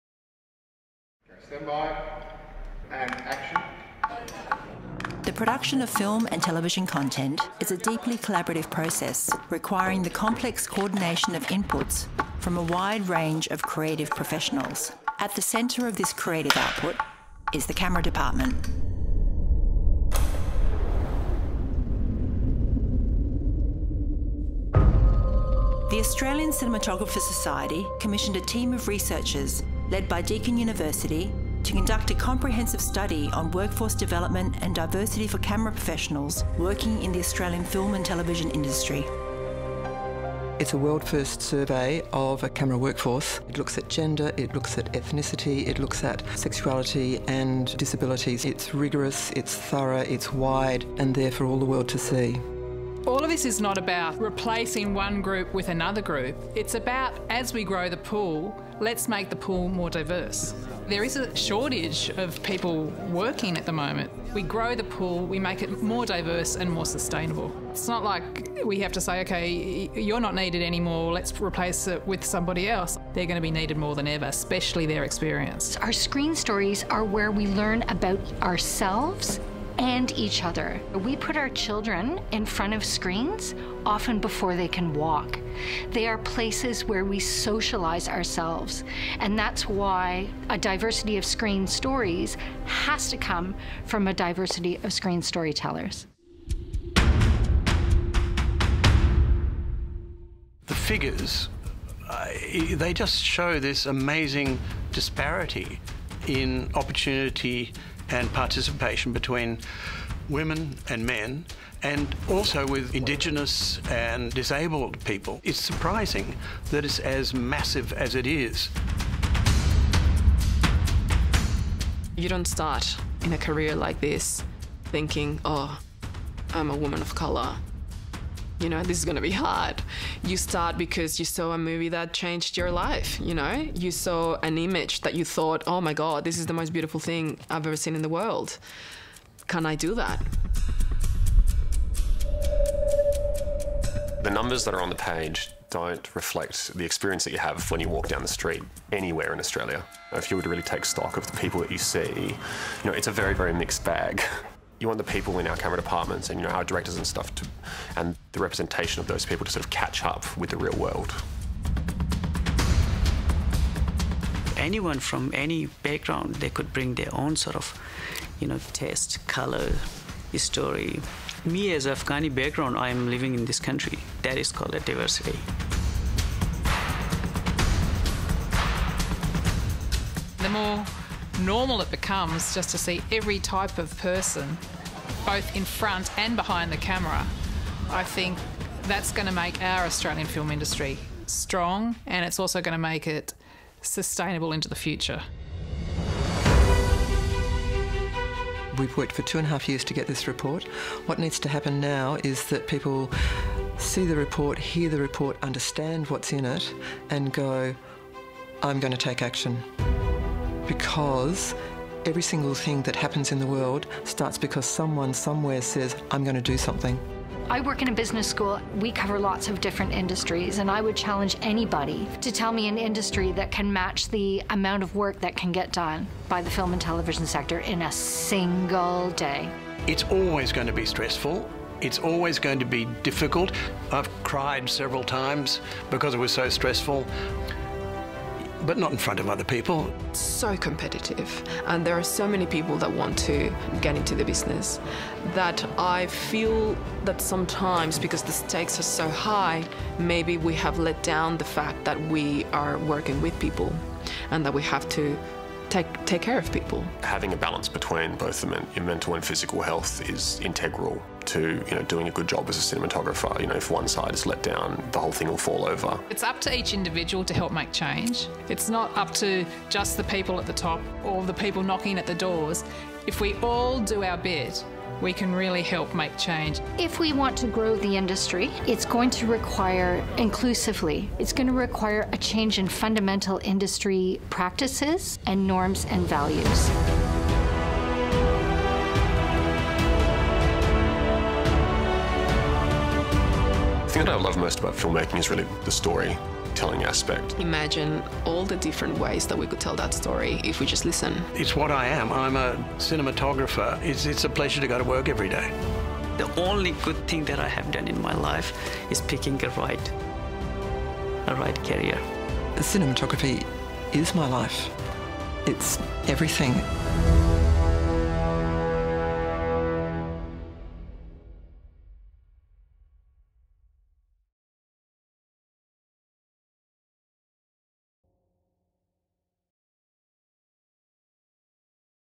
A Wider Lens | Launch Film
The ACS and Deakin University have produced a world first report into Diversity and Development in the Australian Camera Workforce.